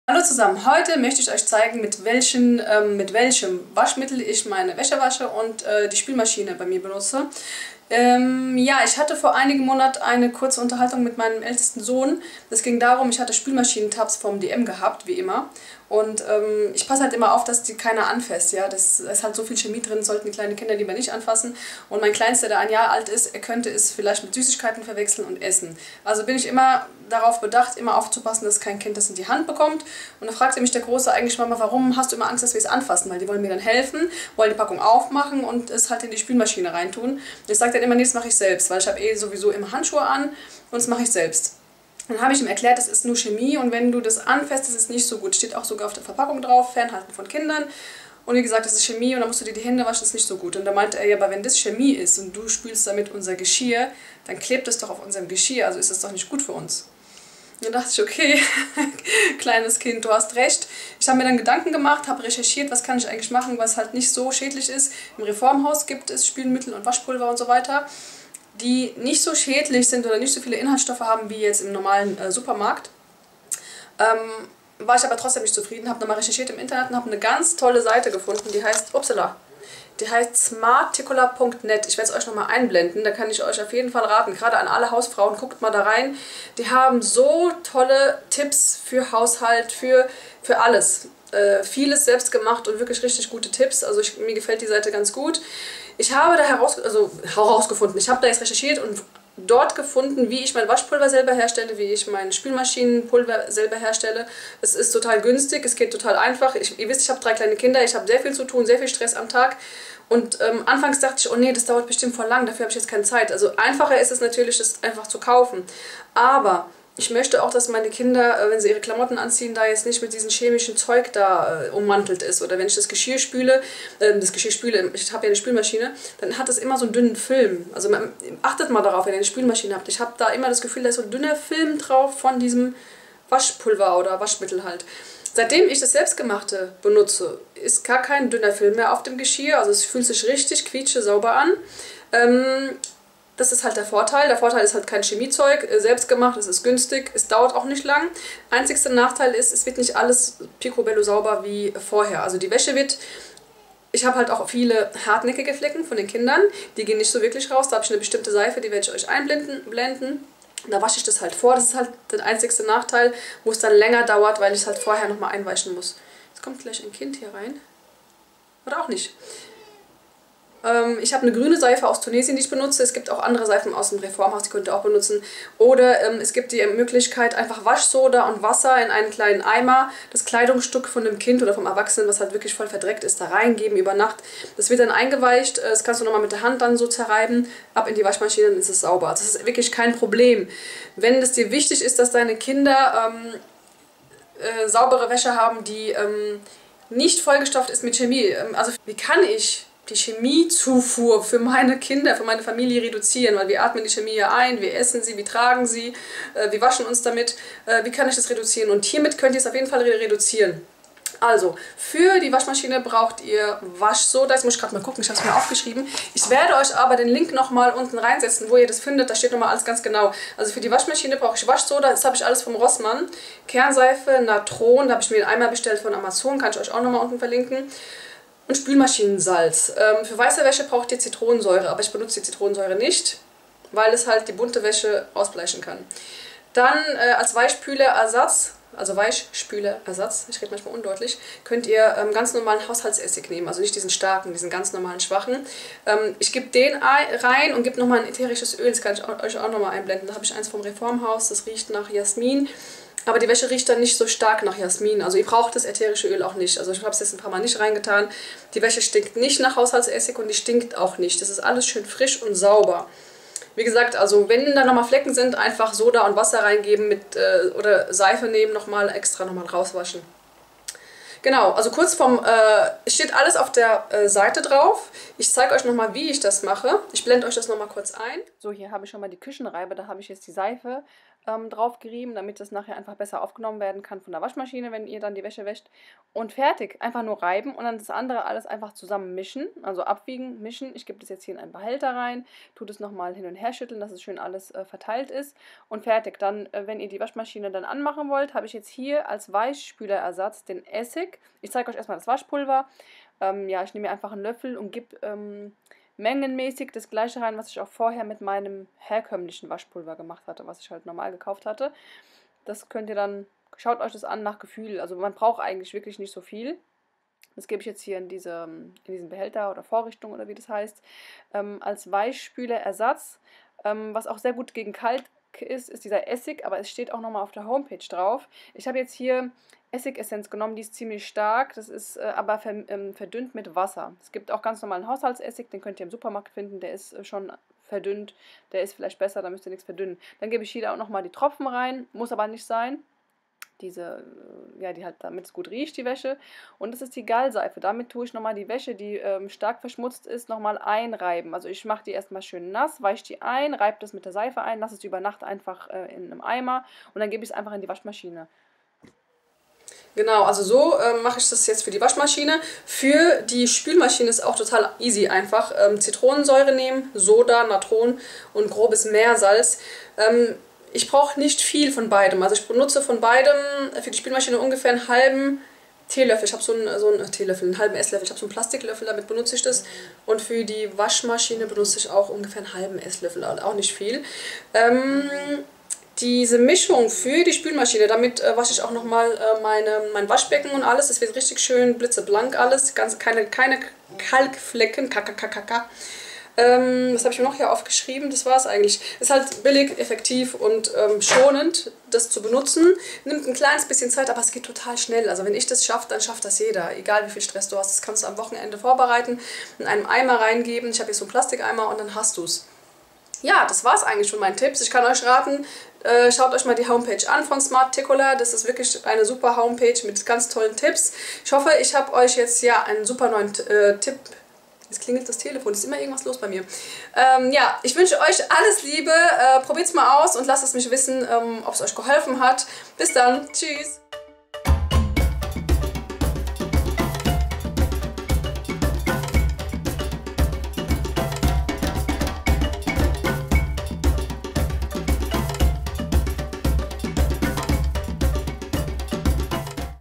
Waschmittel/Pulver Spülmaschine selber günstig herstellen
Erwähnter Blog: smarticular.net

Waschmittel:
100 g Spülmaschinensalz 100 g Kernseife 150 g Soda 150 g Natron

► Kostenlose Tipps? Verpasse kein Video mehr!

Kanal meines Bruders: The Lehrer

*Mein Entsafter

*Unsere Eisform

*Schokoladen Form